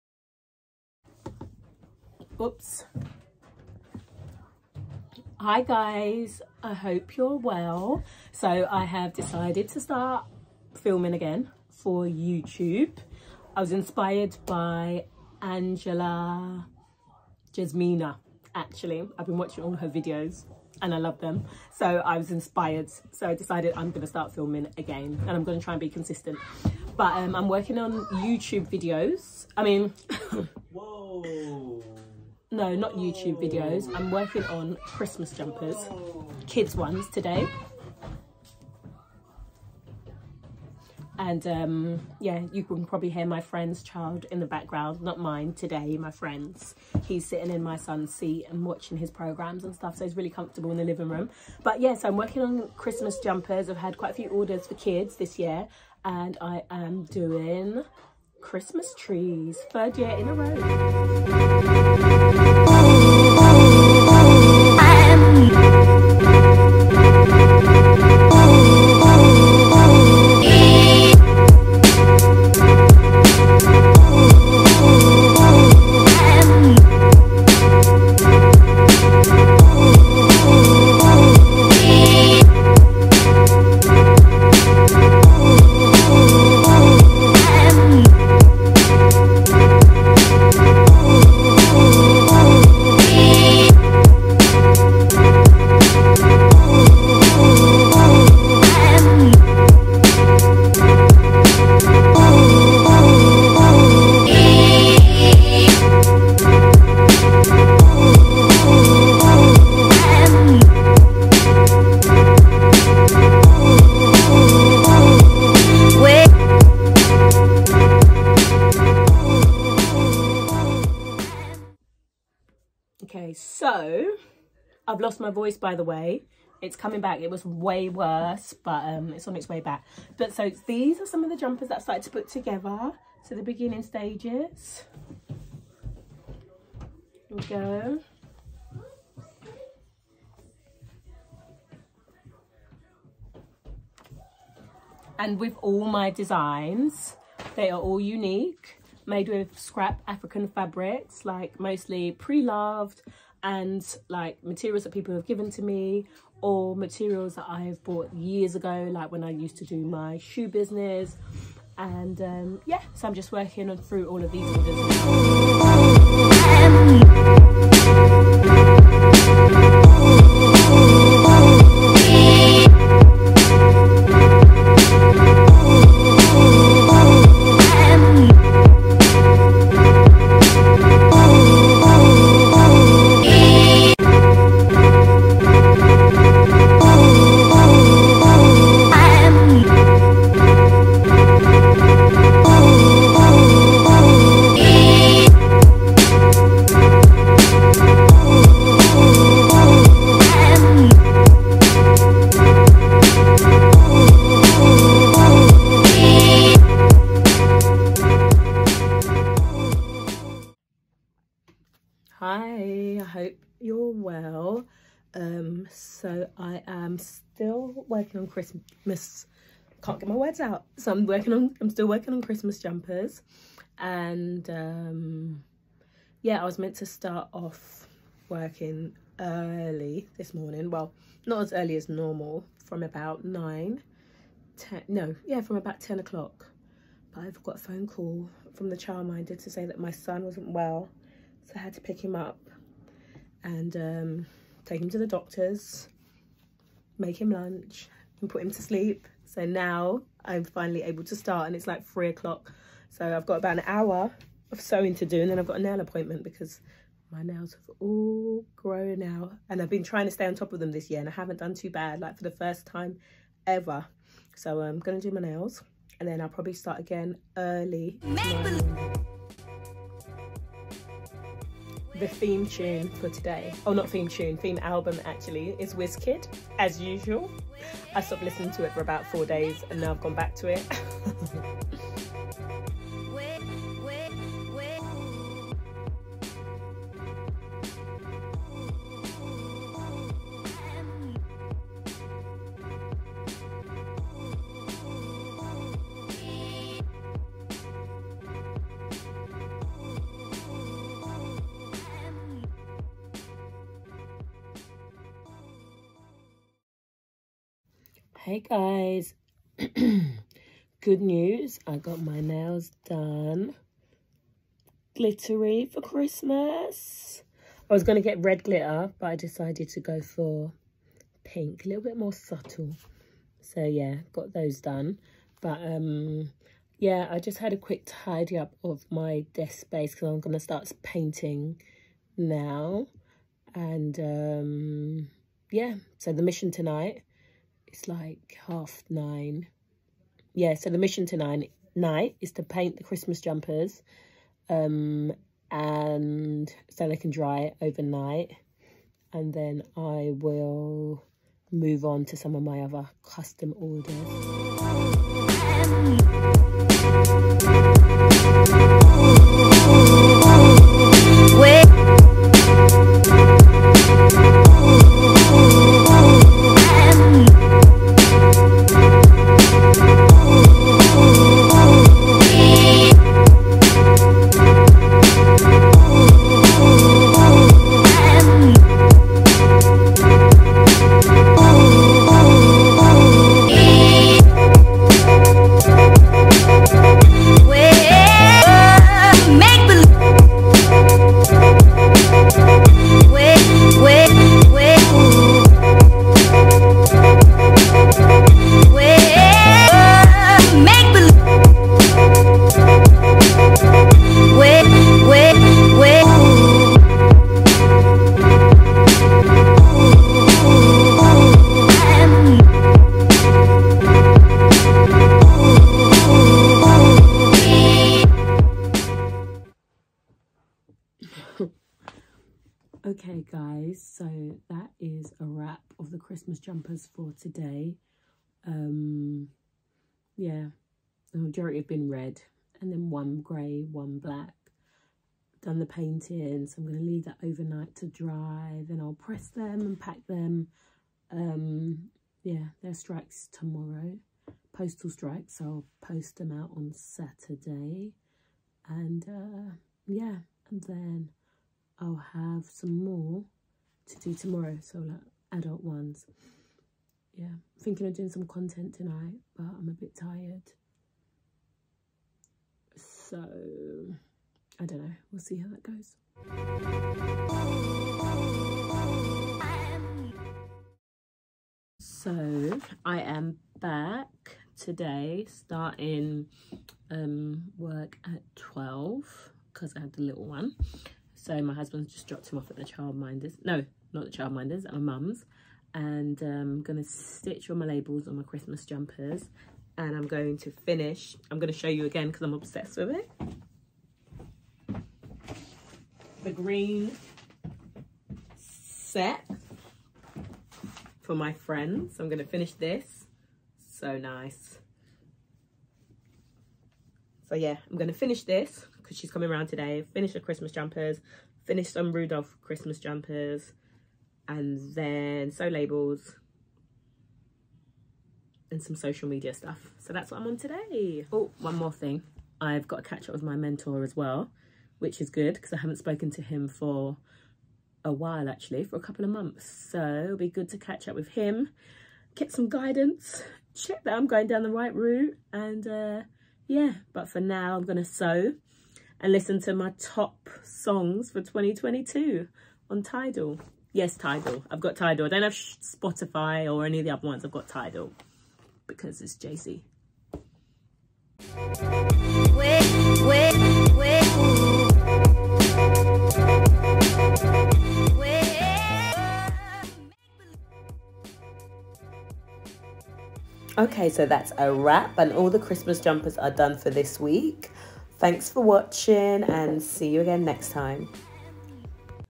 Create Christmas Jumpers With Me
It’s been a while since I’ve posted on you tube. I decided to start again before the year is out and try to be consistent.

In this video you can watch me create my annual Christmas collection. Each jumper is made sustainably using pre-loved clothes and scrap fabrics.

Instagram › dionn...Dionne Gooding (@dionnegooding) • Instagram photos and videos

Thanks
Dionne